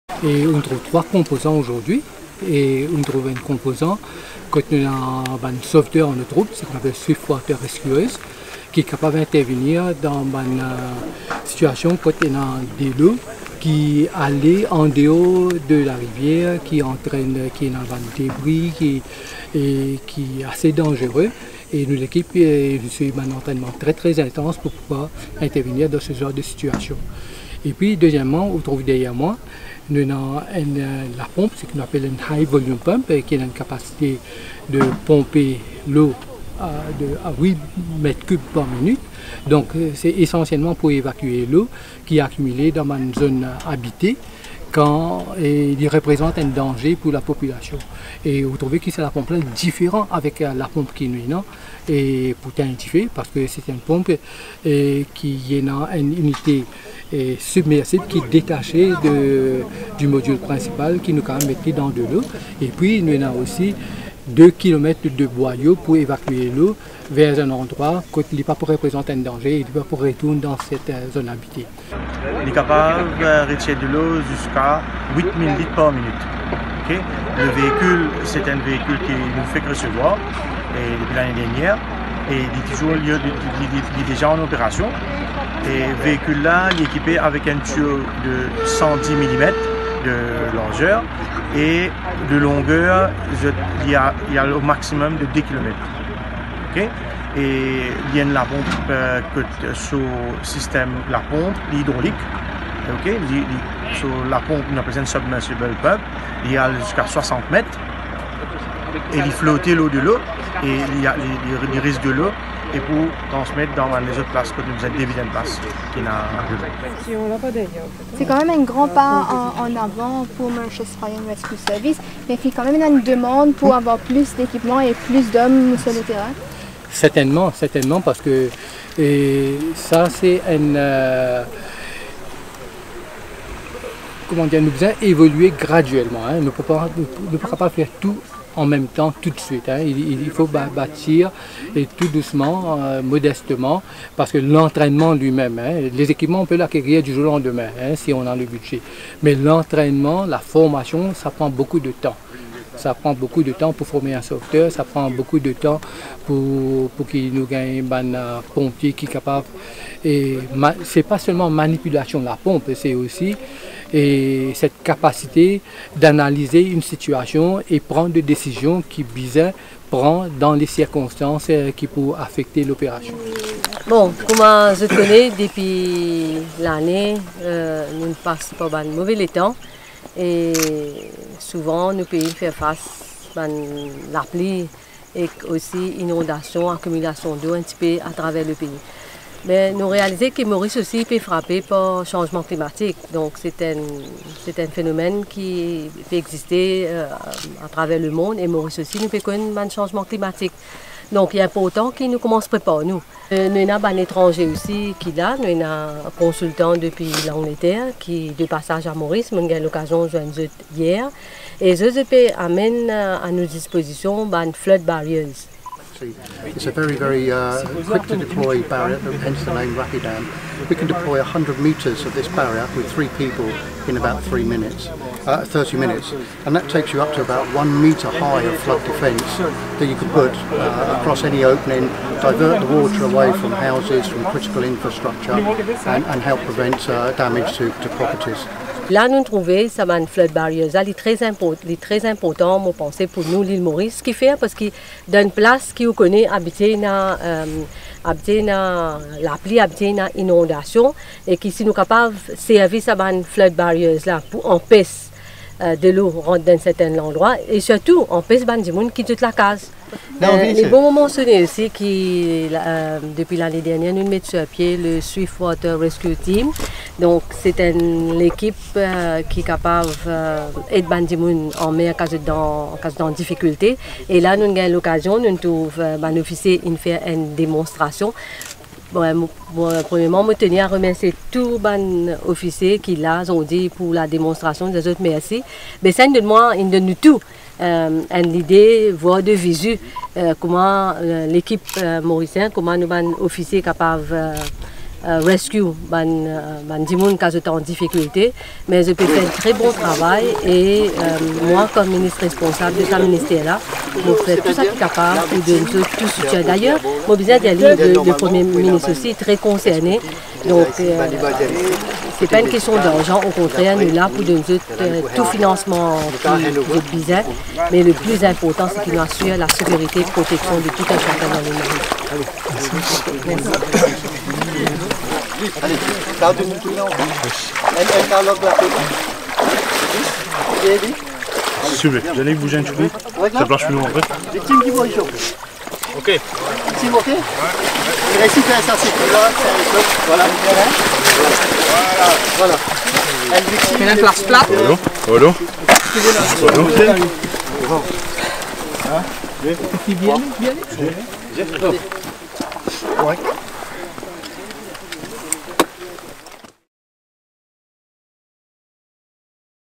Opération sauvetage de la MFRS à Bagatelle Dam
Une opération sauvetage a été effectuée cette semaine par la Mauritius Fire and Rescue Service (MFRS) au barrage du Midlands Dam. Les responsables des pompiers ont eu l'honneur de la présence de la vice-Première ministre afin de démontrer leurs méthodes et les nouveaux équipements qui sont utilisés en cas d'inondations ou de personnes en difficultés en eaux troubles, et par là même, solliciter le support du gourvernement.